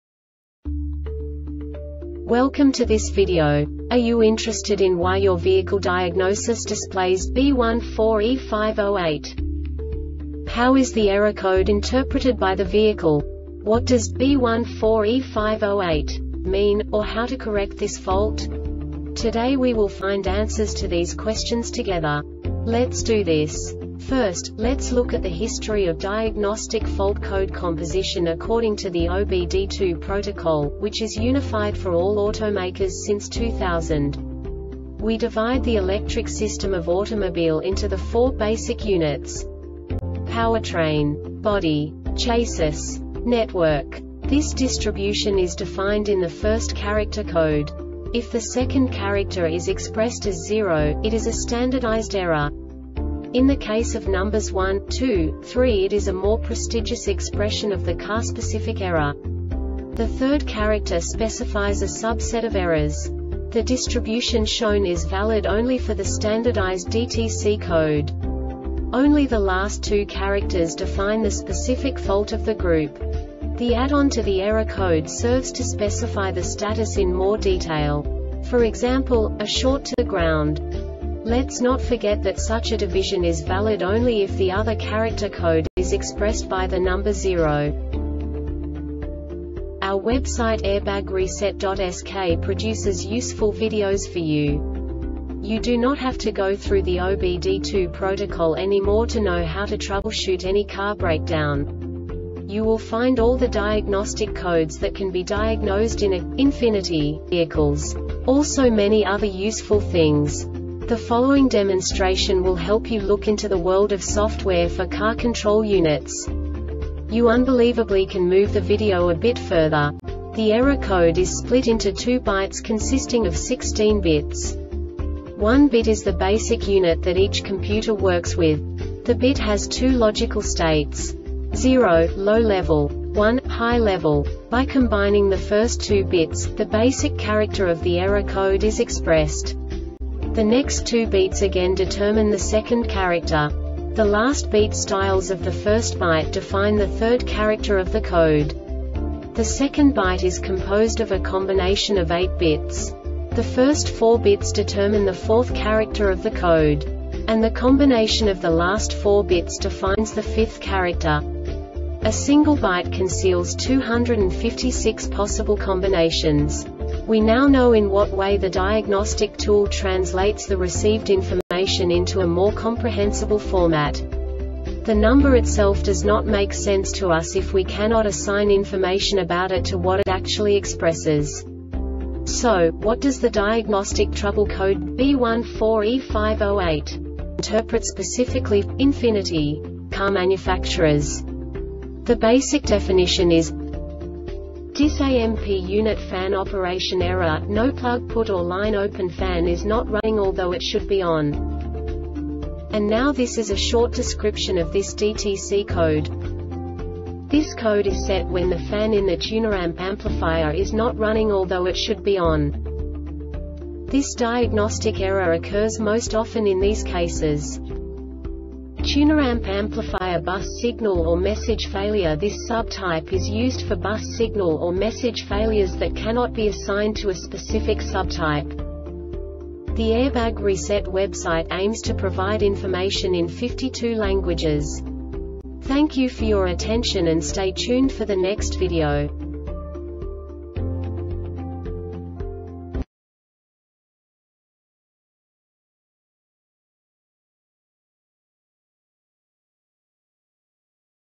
DTC Ford B14E5-08 Short Explanation
Contents:
0:21 Basic DTC analysis according to OBD2 protocol standard.
1:48 Insight into programming
2:58 A diagnostically understandable description of the Diagnostic Trouble Code
3:05 Basic error definition

Make:
Ford
Code:
B14E5 08
Definition:
Description:
Cause:
 Tuner &amp;  Amplifier
Failure Type:
Bus Signal  or  Message Failure
This sub type is used for Bus Signal  or  Message Failures that cannot be assigned to a specific sub type.